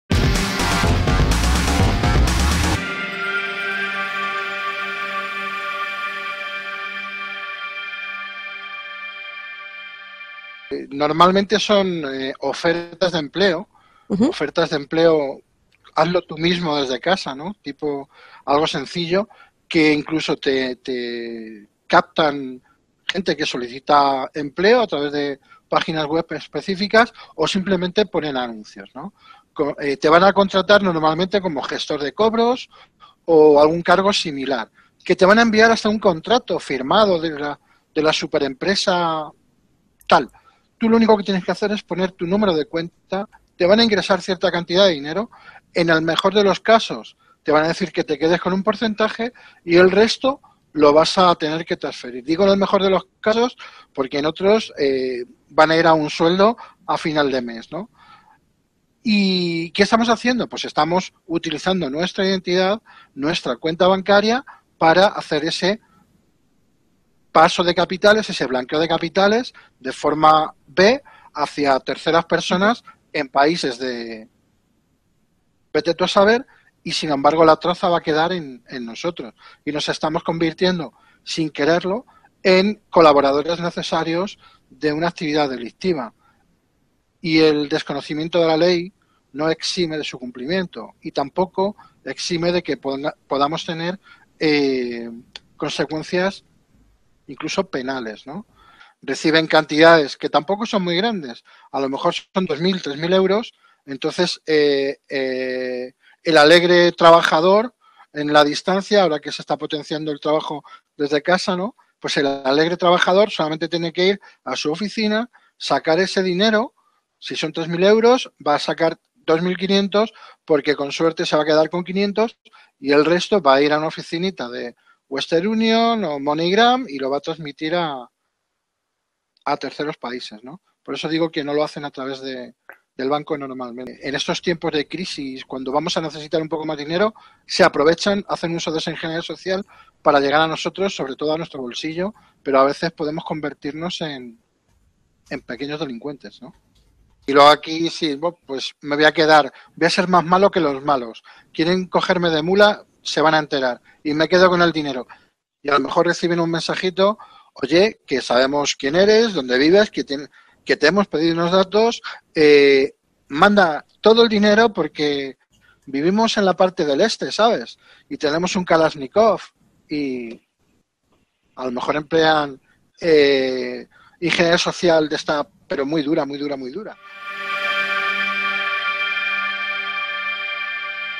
Qué son los muleros bancarios y las consecuencias de esta estafa
Los muleros bancarios dejan de ser víctimas de una estafa para pasar a ser delincuentes por blanqueo de capitales al caer en la trampa de los ciberdelincuentes que los ofrecen 'trabajo' mediante correos engañosos y en tiempos de crisis se aprovechan.  A su paso por Palabra de hacker Ángel Pablo Avilés 'Angelucho' explica en qué consiste ser un mulero bancario y las consecuencias que tiene.

Sigue Palabradehacker tu canal de ciberseguridad de tú a tú